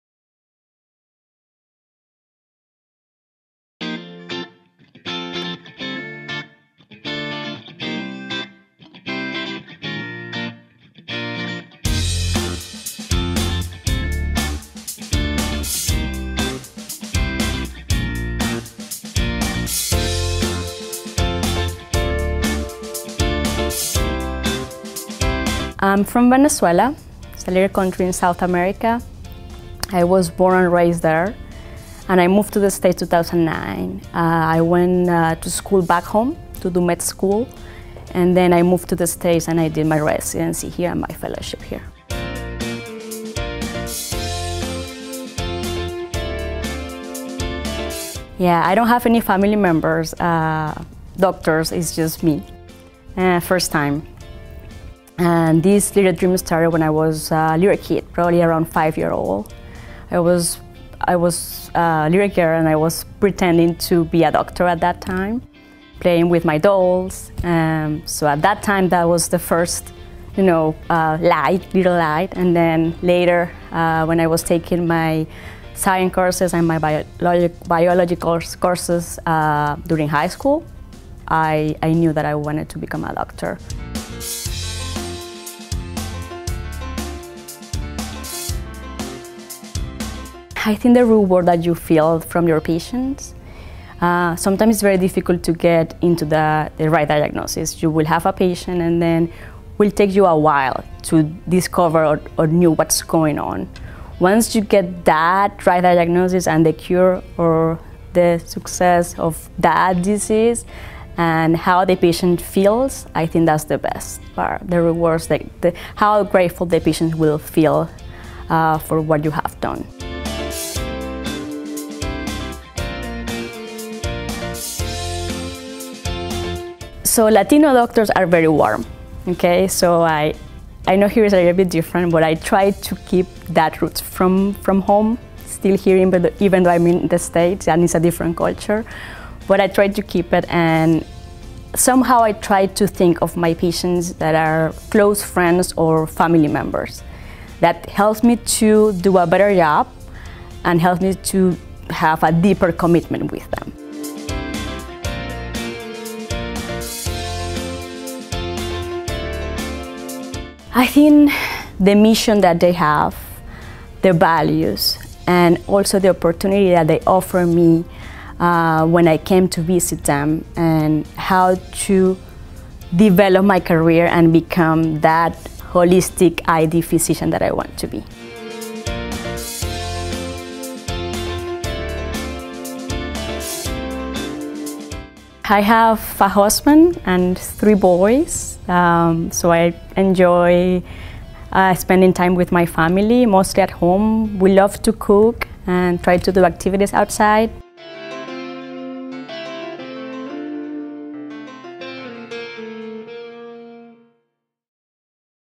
Dayana Maita, MD - Avera Medical Group Infectious Disease Specialists in Sioux Falls, SD
A true detective of medicine, Maita explores diseases, finds roots causes and prescribes appropriate treatments. She treats bacterial, fungal, and viral infections—along with HIV/AIDS and common infections from transplant patients. In her spare time, she loves dining out, doing yoga, watching documentaries and traveling internationally.